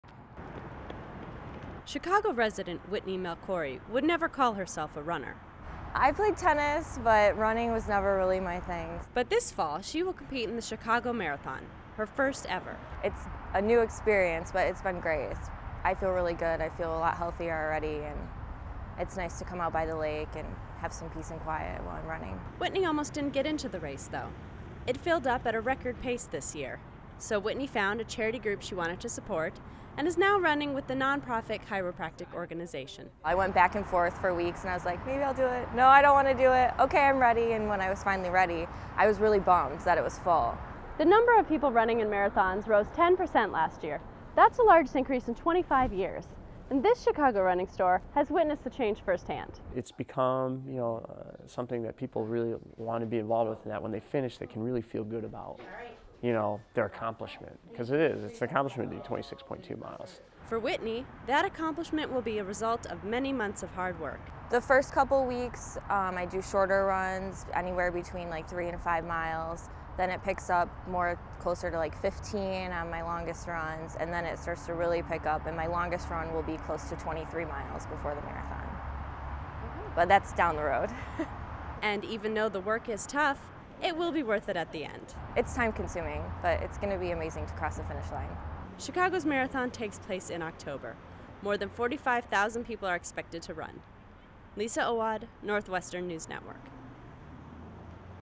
Taking a first run at the marathon
More than ever before Chicagoans are putting on their running shoes to train for a race. Lisa Owad gets us in step with this rising trend.